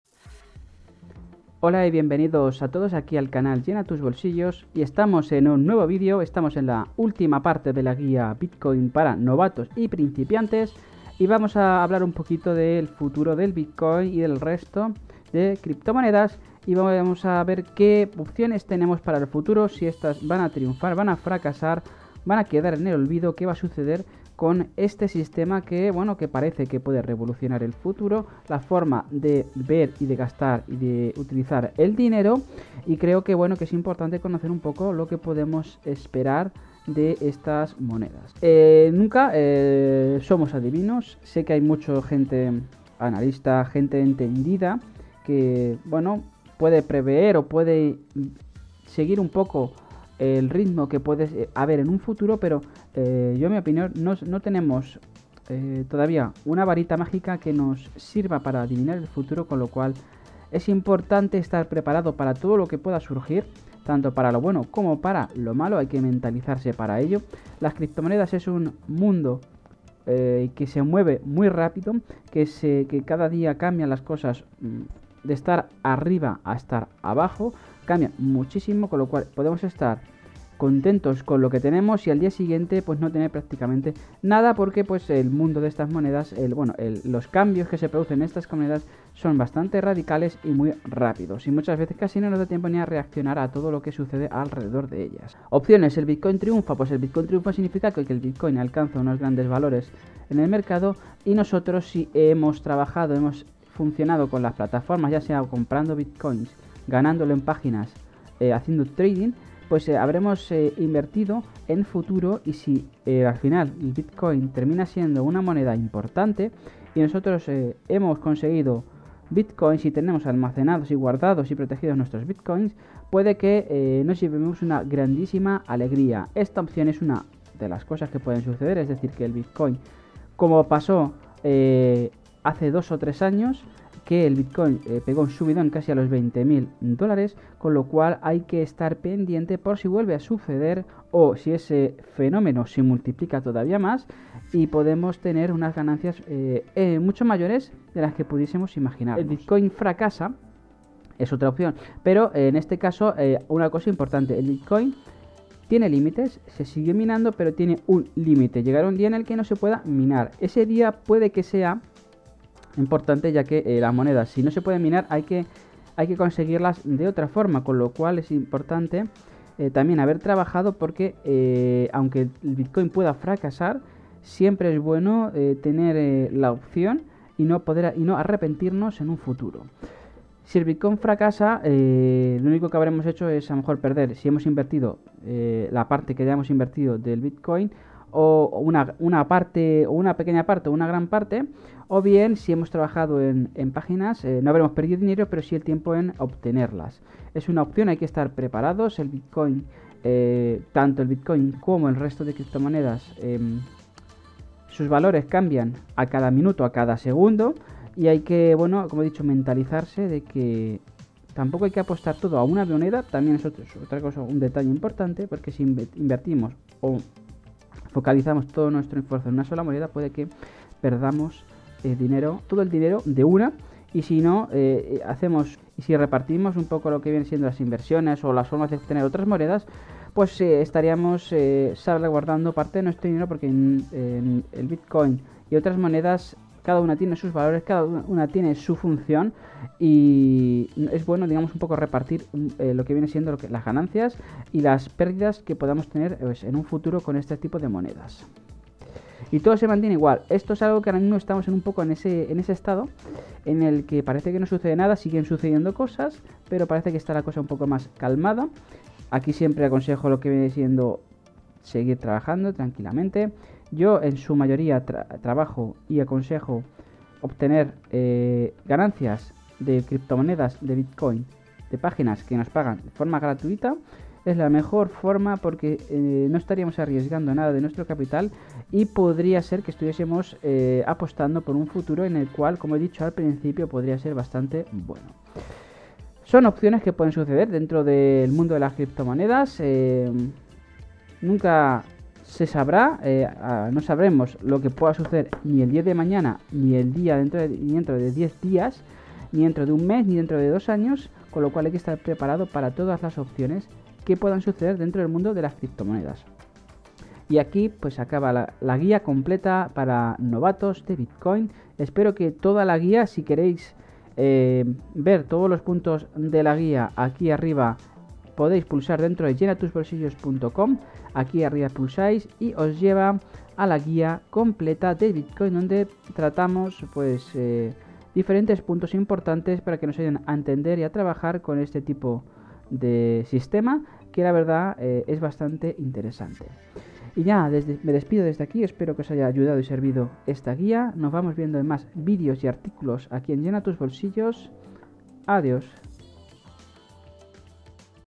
Guía Bitcoin para novatos - 10 - El futuro del Bitcoin
🔴  NUEVO VÍDEO   🔴

Último apartado de la guía de Bitcoin para novatos y principiantes donde vamos hablar sobre le futuro del BTC y que opciones tenemos.

Hay que estar bien informados y estar preparados para todo lo que vaya a suceder.

● Llenatusbolsillos.com